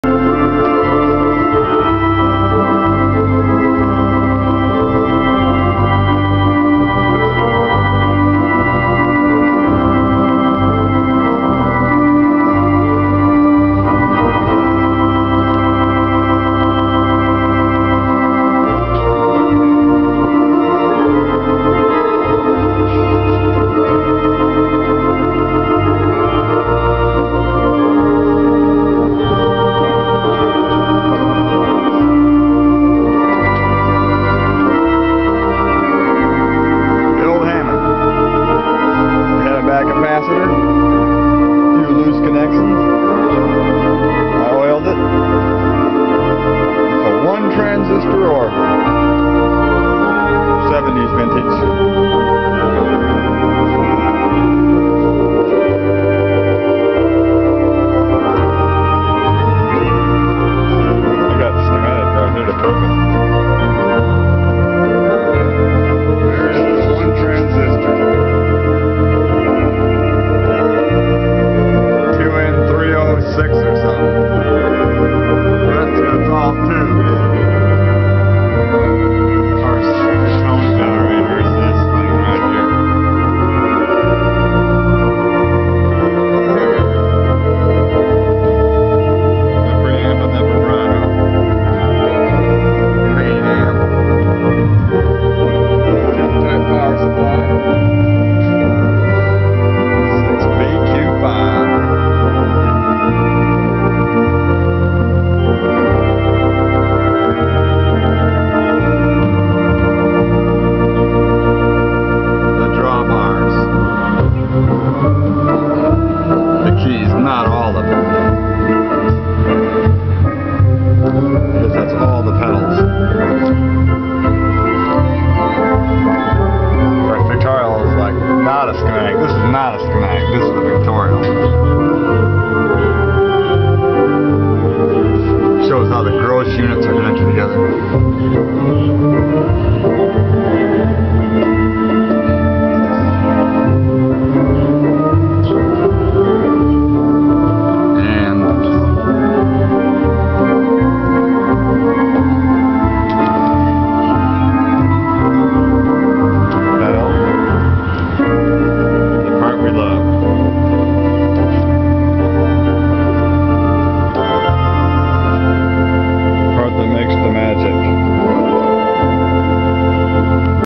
Hammond TONE WHEEL organ
A  Hammond organ has a mechanical tone generator mechanism (tone wheels) and is somewhat like a old style clock. It is full of gears connected to a motor to turn them. Each gear has different number of teath to make a different note. Like a clock it needs to be oiled. Do yourself a favor and use the correct type oil.